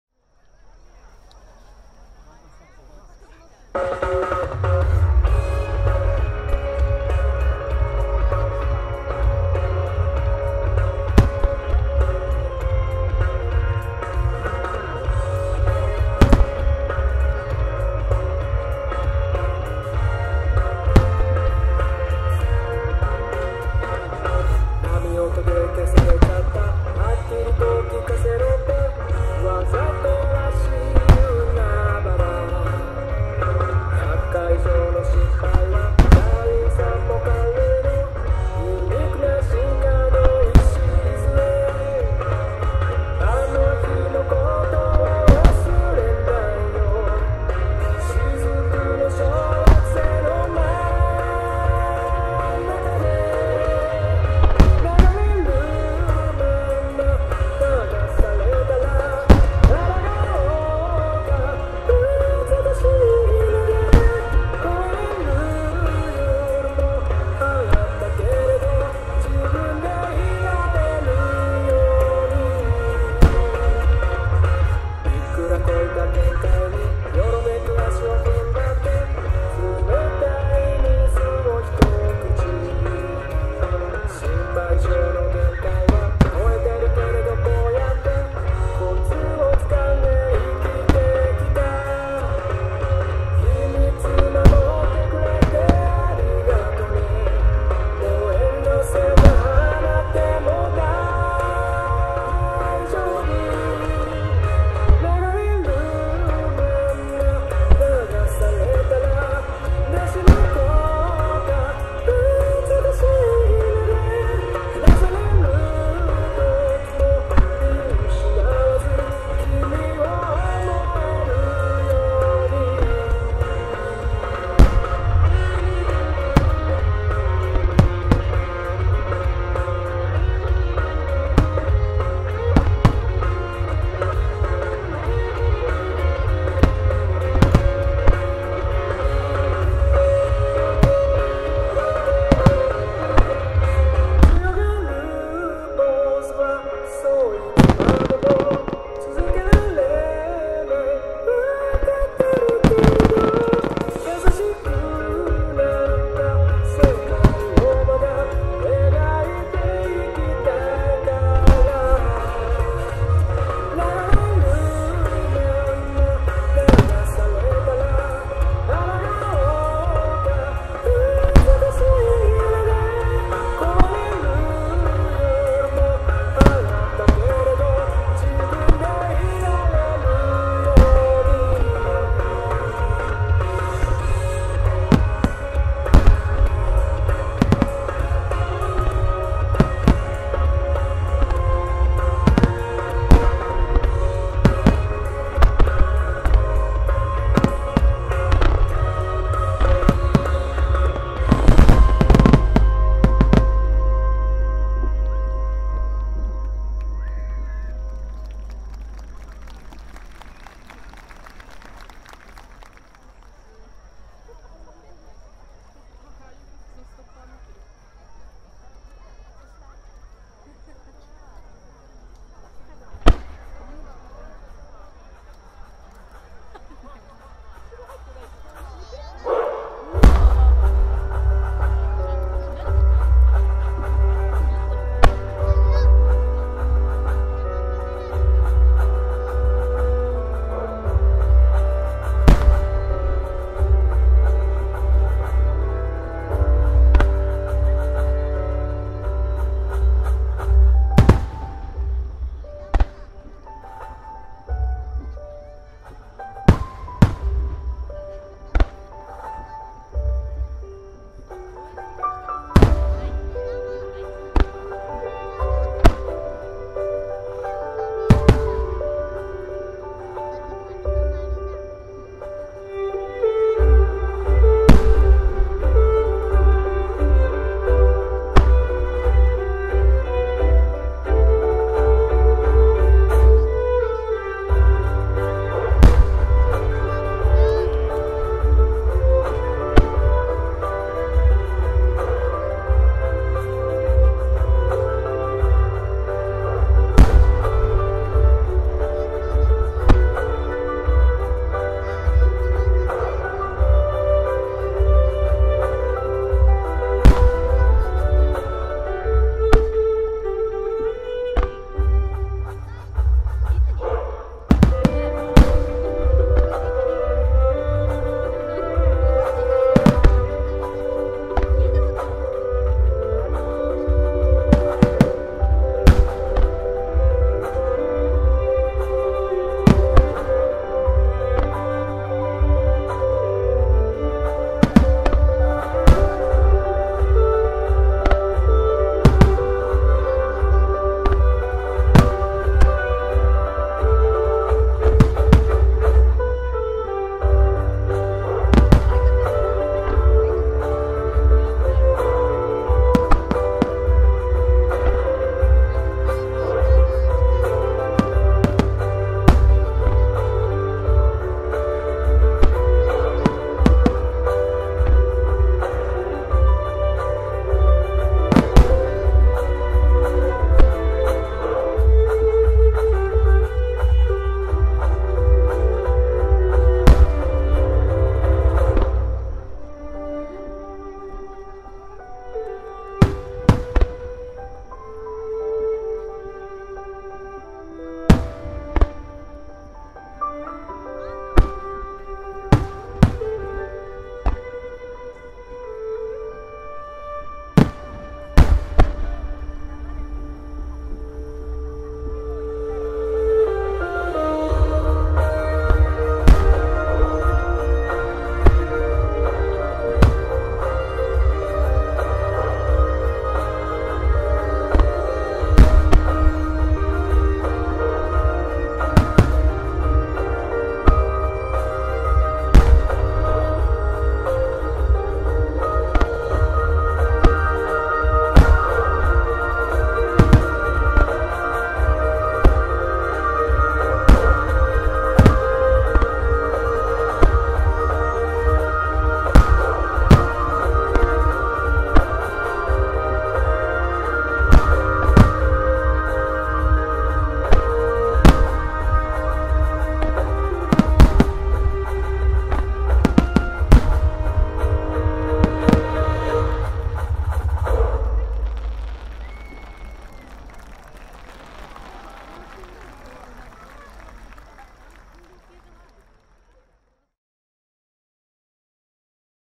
【茨城県境町】【2023】第36回利根川大花火大会 ハナビのチカラ 大玉花火に願いを込めて 「美しいFIRE ー花火進化論ー【㈱山崎煙火製造所】」「大輪の花【野村花火工業㈱】」
Tonegawa River Fireworks

2023年9月16日㈯
茨城県猿島郡境町

花火業者：㈱山崎煙火製造所・野村花火工業㈱
うｐ主 X：LDK_T​​​